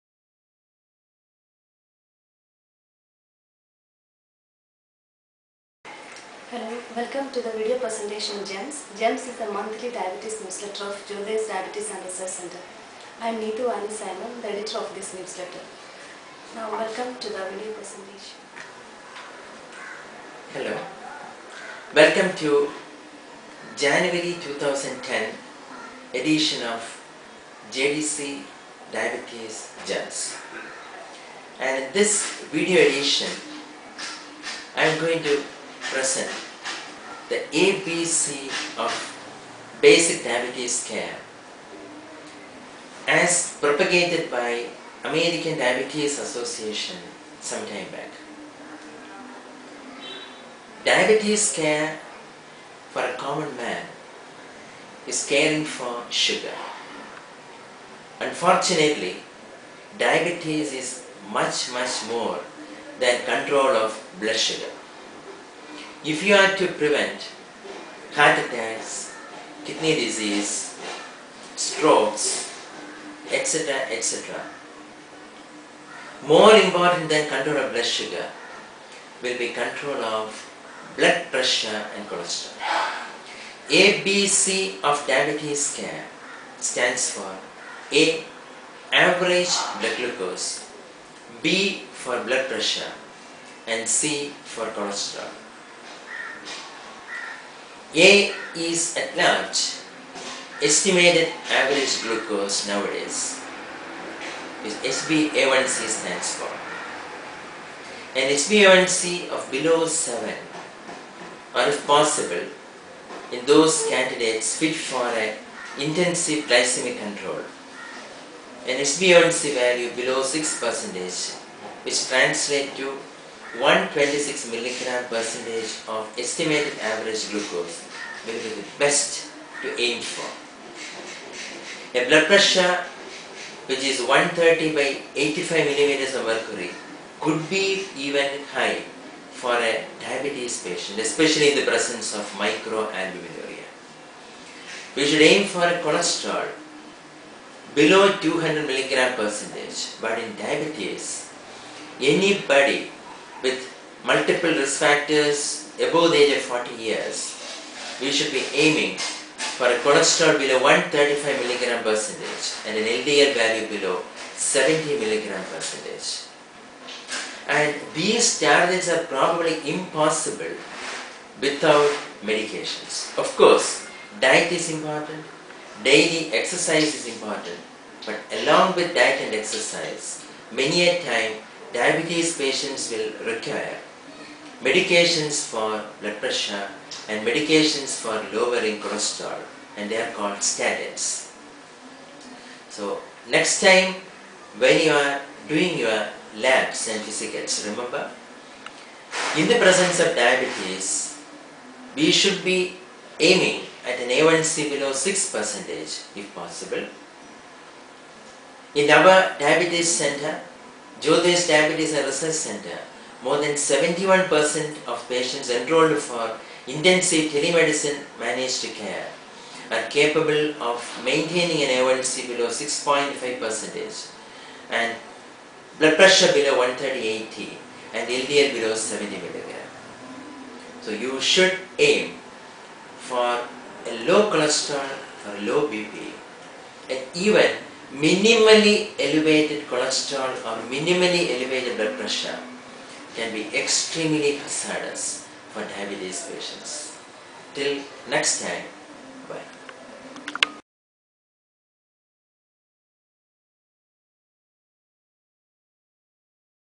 gems003("ABC of Diabetes Care" by Jothydev)
Dr.Jothydev Kesavadev speaks on "ABC of Diabetes Care" : JDC diabetes Gems video edition